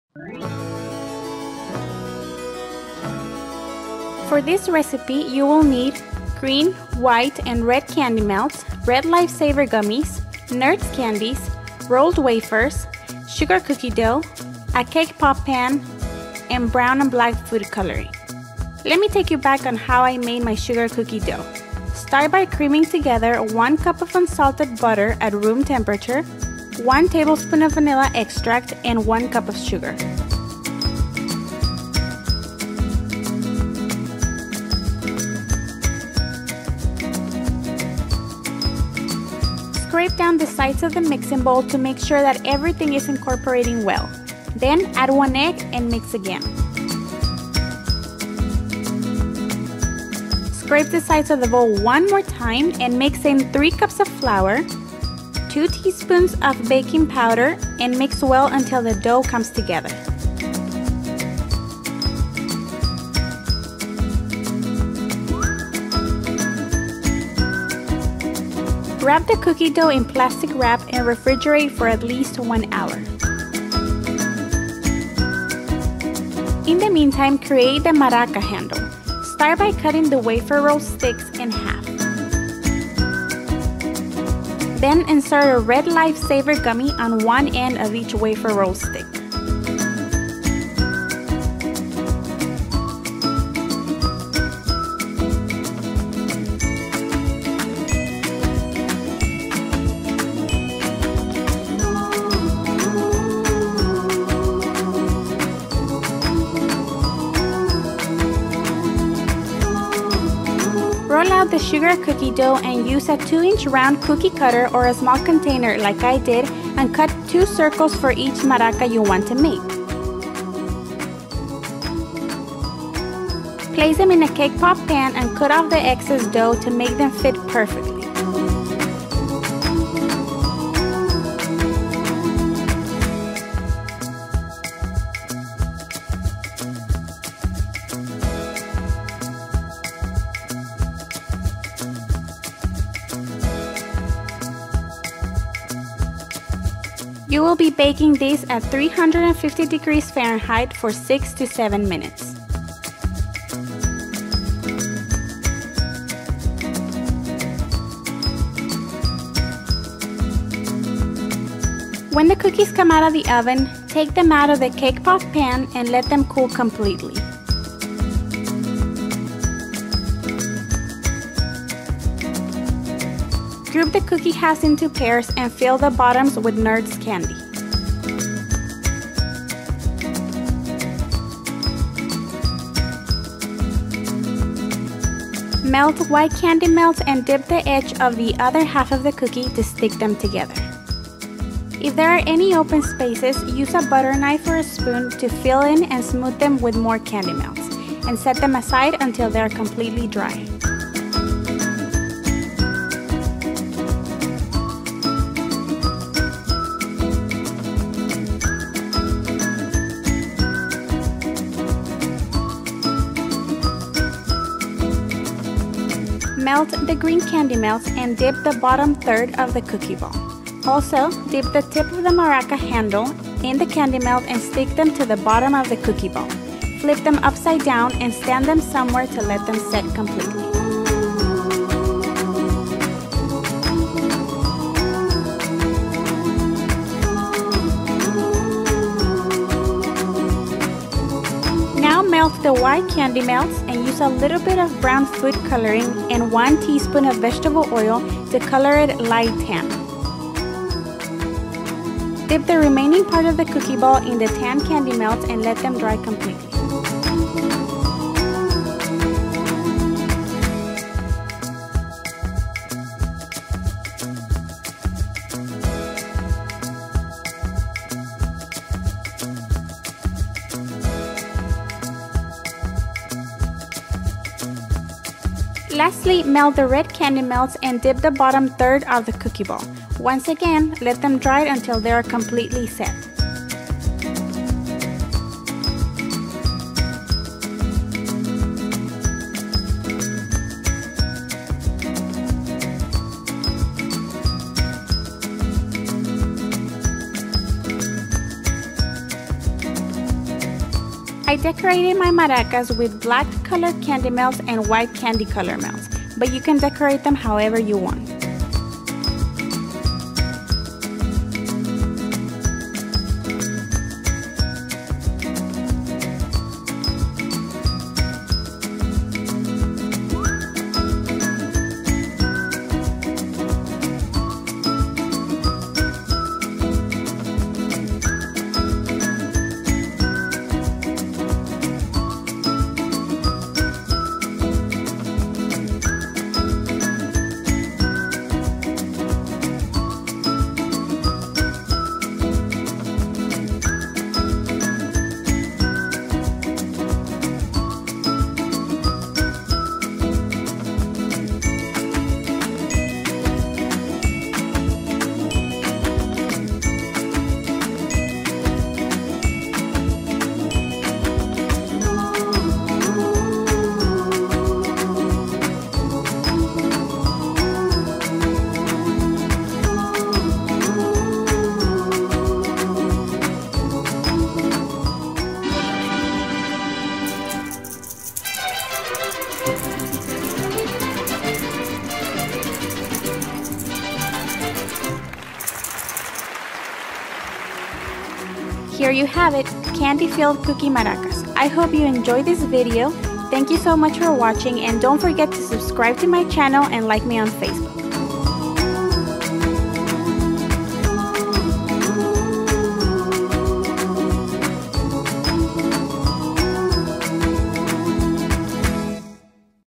Candy Filled Cookie Maracas
5 de mayo is coming up and i thought this maracas were perfect for the occasion, they were also really fun to make. I got the idea from pinterest, but as we all know, pinterest does not always do a good job with instructions. I changed a lot of things to make them as easy as i could, and added the sugar cookie recipe.

INGREDIENTS:

Green, White, and Red candy melts
Red life saver gummies
Nerds candy
Cake pop pan
Brown and Black food Coloring
Sugar cookie dough

 FOR THE SUGAR COOKIE DOUGH:

1 cup of unsalted butter at room temperature
1 tablespoon of vanilla extract
1 cup of sugar
1 egg
3 cups of flour
2 teaspoons of baking powder

INSTRUCTIONS:

FOR SUGAR COOKIE DOUGH:

1. Cream together butter, vanilla extract and sugar. Scrape down the sides of the bowl and mix in one egg. Scrape the sides of the bowl again and mix in the flour and the baking powder. Mix until it comes together, wrap the dough in plastic wrap and refrigerate it for one hour.

FOR MARACAS:

1. Create the maraca handle by cutting the wafer roll sticks in half, then stick one red life saver gummy at one end of each stick
2. Roll out the cookie dough and use a 2 inch round cookie cutter or a small 2 inch round container and cut 2 circles per one maraca you will make.
 (This amount of dough will make 3-4 dozens of maracas. I didn't make a lo of them so what i did is wrap my dough in plastic wrap and freeze it to use it on regular sugar cookies later. Also you could cut the recipe in half if you don't want to have all that cookie dough left)
3. Place one dough circle inside each crevice of the cake pop pan, smooth it out so it can fit perfectly and cut off the excess from the top if needed. Bake them for 6-7 minutes at 350 degrees fahrenheit. When they come out of the oven, take them out of the pan and let them cool completely
(If when the cookies come out of the oven,  they have a few air pockets just take a little spoon and lightly press down the air pockets with the back of the spoon to smooth them down)
4. Group the cookie halves in pairs and fill the bottom halves with nerds candies. Melt the white candy melts and dip the top halves in the candy melts and stick them together with the bottom halves. Use a butter knife and more candy melts to fill in the open spaces. Put them aside until the candy melts are dry.
5. Melt the green candy melts and dip the bottom third of the cookie ball. Also dip the maraca handle (the side that has the gummy) and stick it to the maraca ball. Let that set.
6. Melt the white candy melts and add 1 teaspoon of vegetable oil per 1/2 cup of candy melts and a little bit of brown food coloring to color it light tan. ( the vegetable oil helps the candy melts not to dry up when adding the food coloring)
DIp the remaining of the cookie ball in the tan candy melts and place them somewhere to set completely.
7. Lastly melt the red candy melts and dip the top third of the cookie ball and once again let them dry completely.
8. I decorated mine with white candy melts and red candy melts colored black also using vegetable oil. You could buy black candy melts but since the amount you will use is very little i decided to just color mine black. The reason why i colored the red ones instead of the white ones is because i have tried before and when i try coloring the white candy melts black it never gets to that color even if i add the whole jar of food coloring, it gets to grey and no matter how much vegetable oil i add it dries up. SInce red is already dark it requires less food coloring to achieve that deep black color.
9. Decorate the maracas how ever you like, you can also just color your maracas one color instead of three , you decide.
10. Shake them and eat them :D